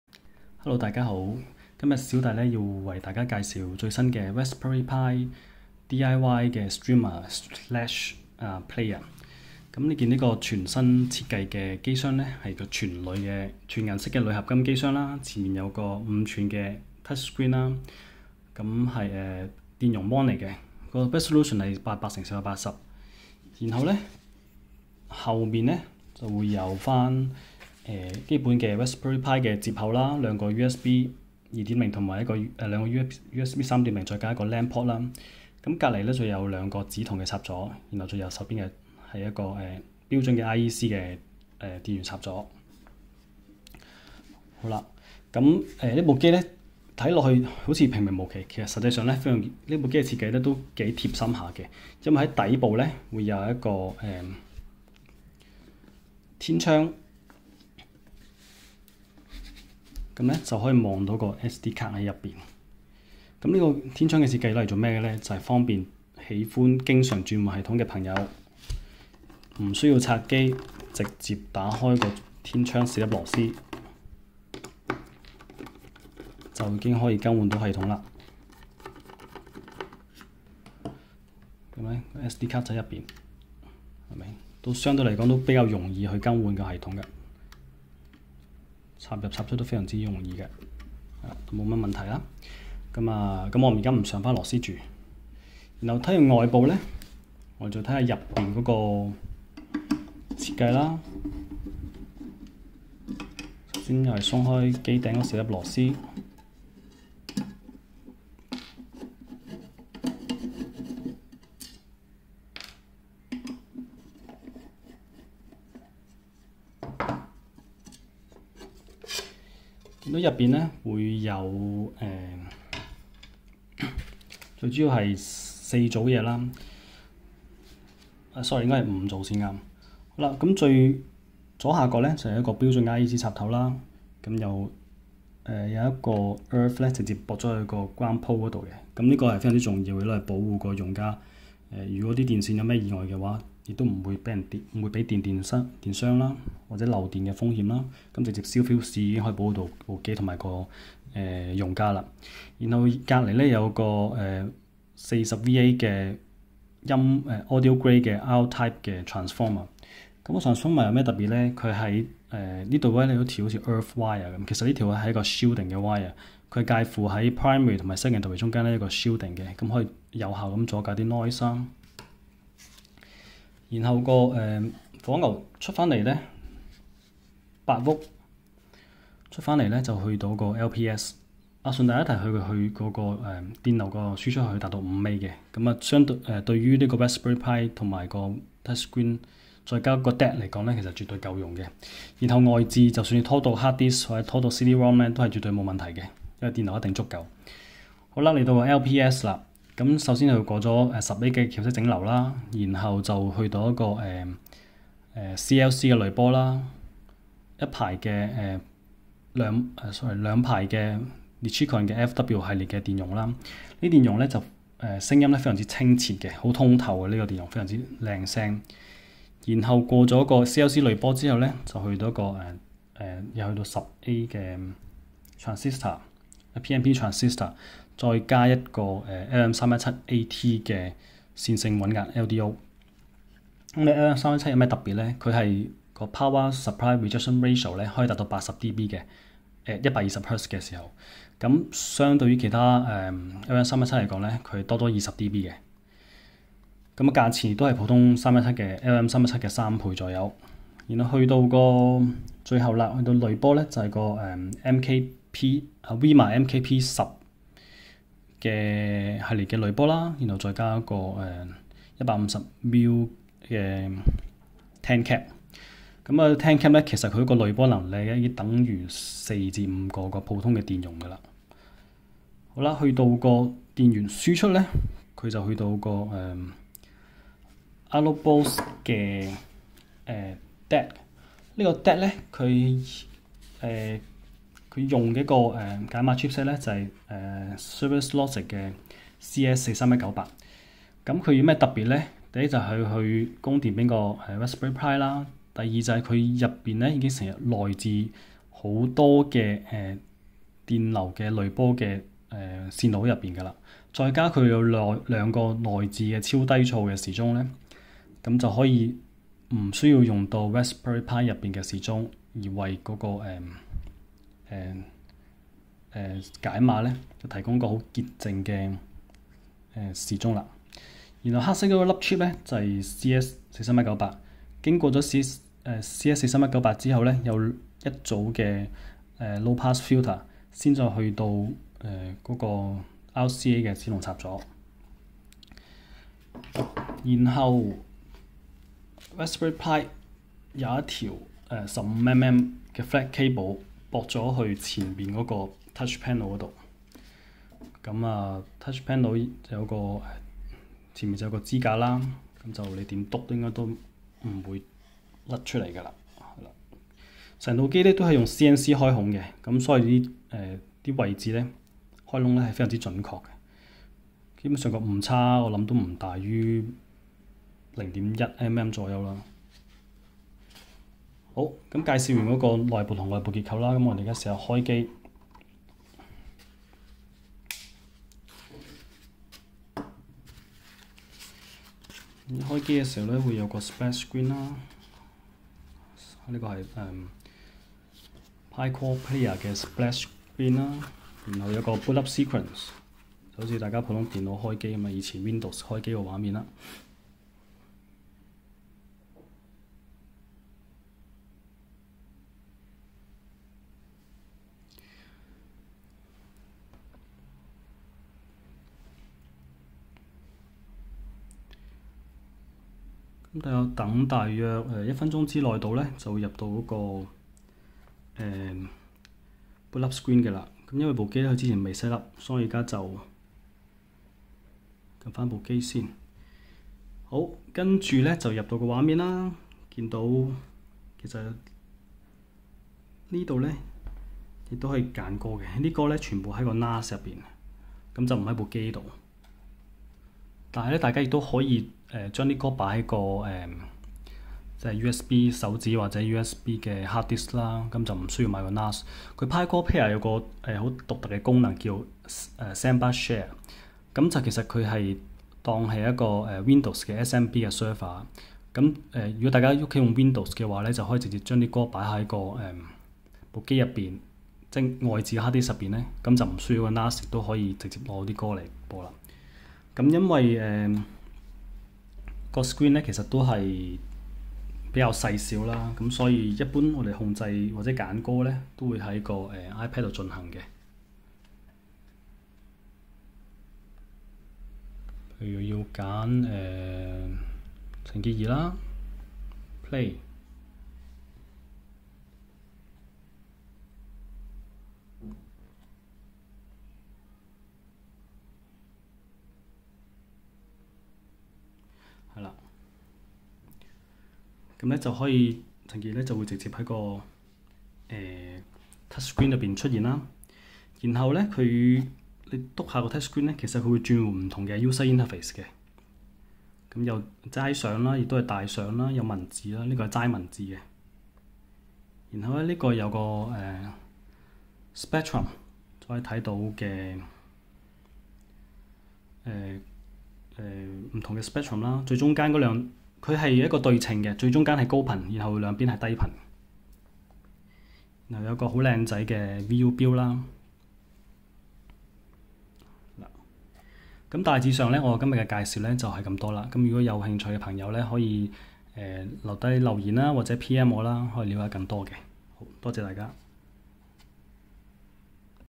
Raspberry Pi Streamer / Player Demo
Introduction to the DIY Raspberry Pi Streamer / Player (with Allo Boss2 CS43198).

Case：
-Aluminum alloy fabricated with CNC and LASER cut
-Opening at the bottom, can change OS with ease
-Dimensions:215(W) x 100(H) x 245(D)
Power:
-40VA audio grade R-type transformer, 5A output
-Built-in LPS, 0.1% load regulation， 80dB PSRR (120Hz)
-8x Nichicon FW Series capacitors
-Ultra low ESR，CLC filtering topology，WIMA MKP10 and TAN CAP for final decoupling
System：
-5“ 800x480 touch screen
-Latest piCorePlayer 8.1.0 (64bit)
-Enhanced system, replaced with NotoSansCKJ, supports Asian characters
-Main processor is Raspberry Pi 4B (4GB)
DAC：
-Allo Boss2 Cirrus Logic CS43198 32bit/384kHz, supports DoP64
-Built-in dual ultra low noise NDK SDA clocks
-THD+N -114dB
-Copper RCA sockets
Functions:
-Control playback via Logitech Media Server
-Supports NAS (NFS或CIFS protocols)
-Supports Spotify Connect (control via apps）
-Supports Tidal (control via web, limited to premium users only)
-Supports external DAC
-Supports external USB hard drive (Up to 2TB)
-Supports external USB CD playback
-Supports CD rippling (limited to Volumio premium users only)